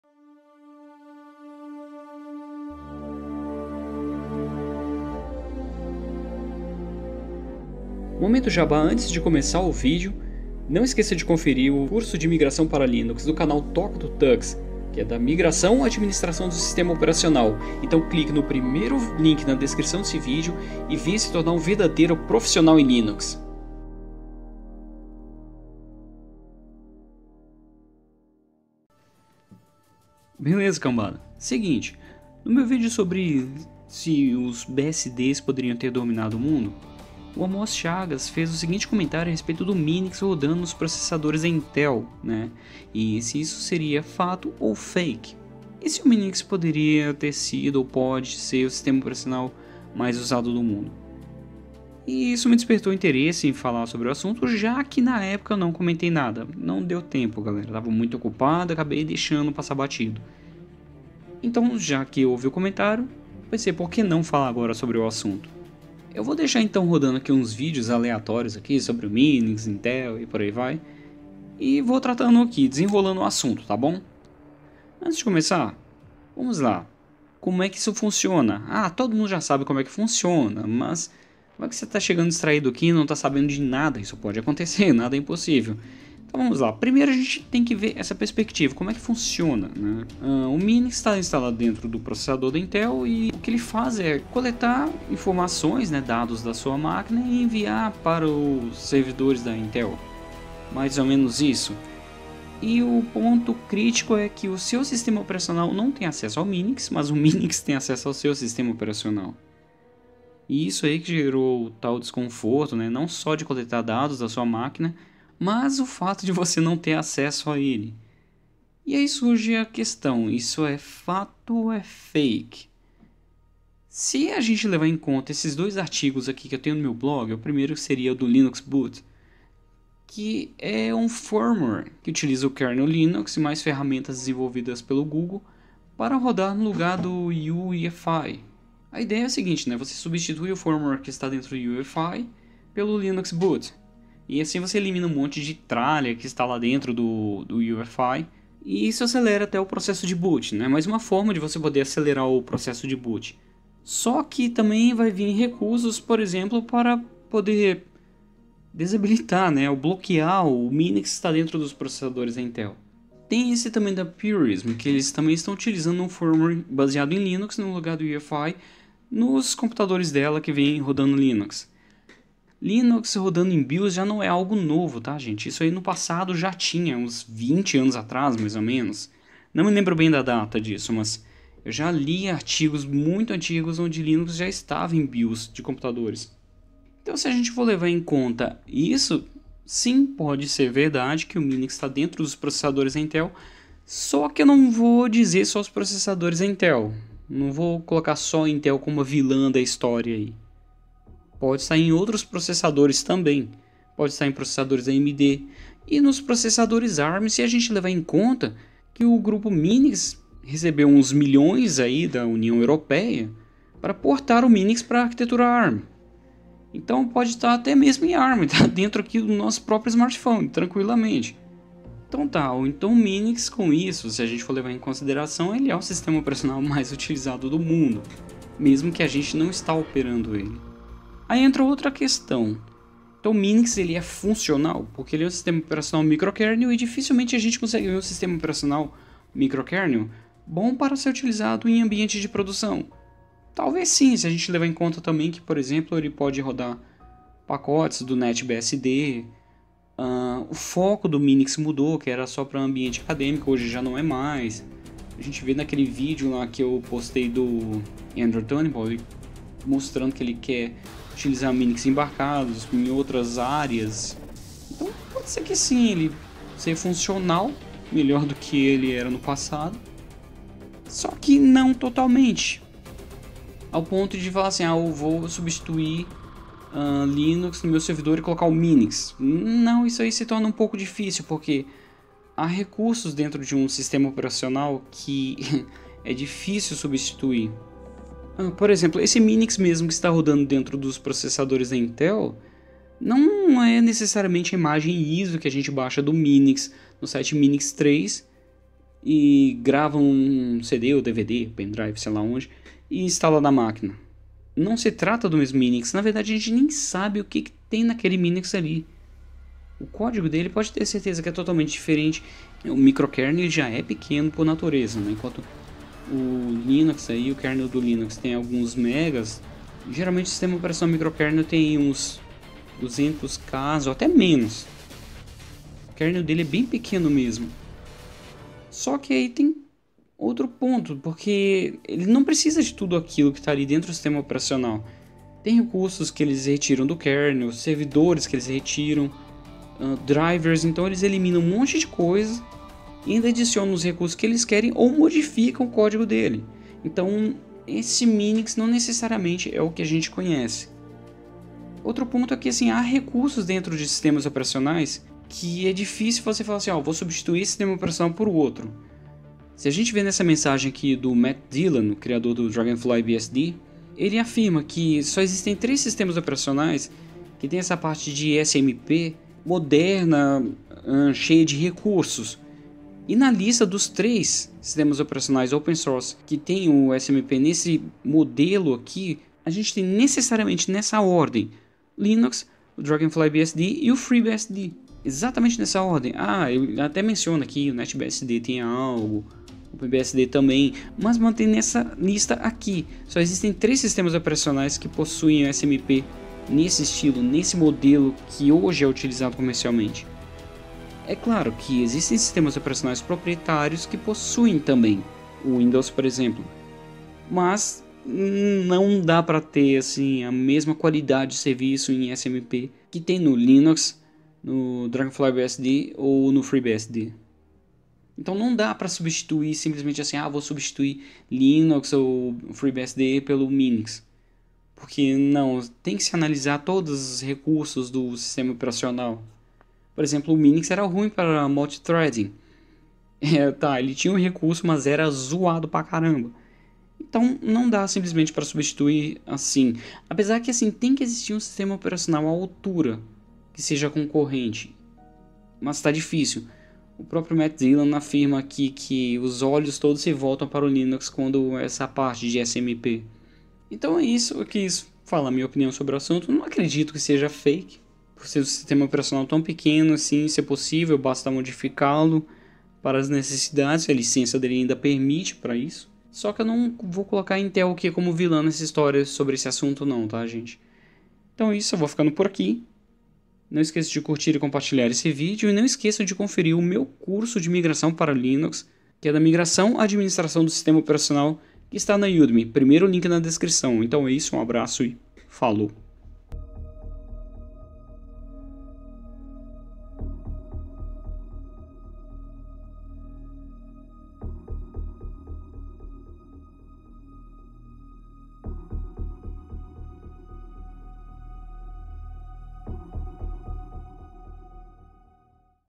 Minix nos processadores da Intel Fake ou fato?
VENHA APRENDER LINUX COMIGO E TORNE-SE UM PROFISSIONAL DE VERDADE

O sistema operacional Minix foi encontrado dentro dos processadores da Intel, mas será que é verdade mesmo?

Linux®